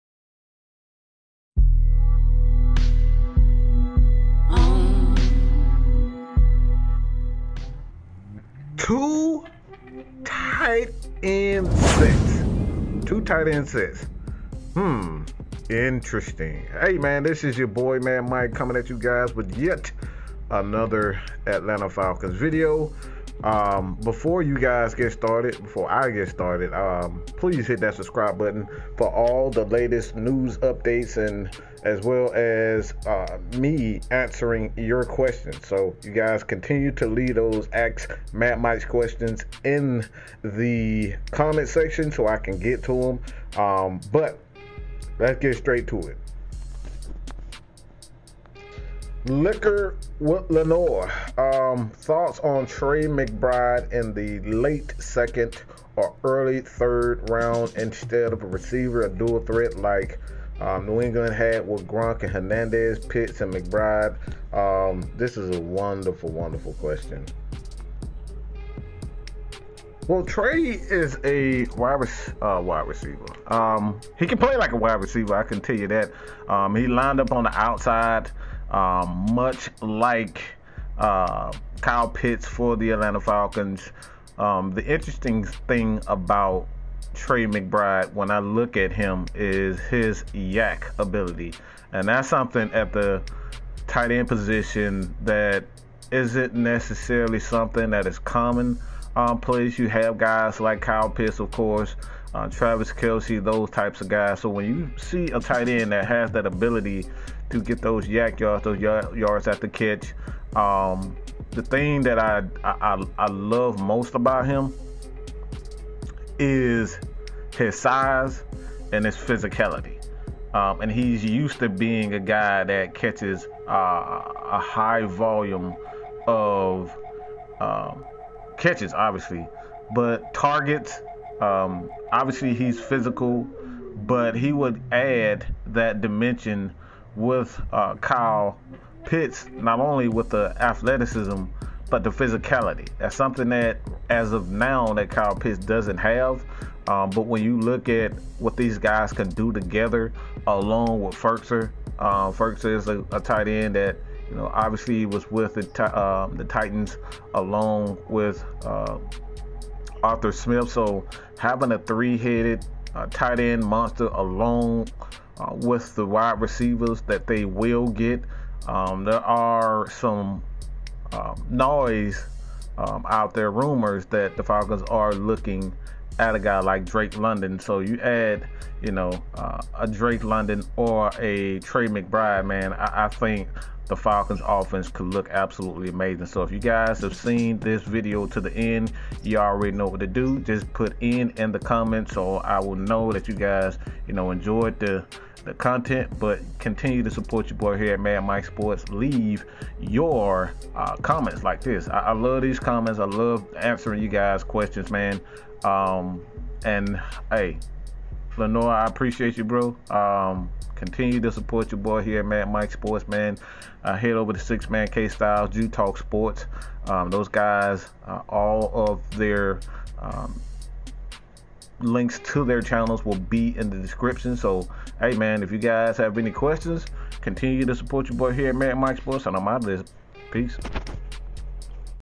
Should The Atlanta Falcons Draft Trey McBride to team with Kyle Pitts? (Ask Mad Mike)
Atlanta Falcons Draft 2022 Prospect question for your boy Mad Mike from Liquor by Lenior.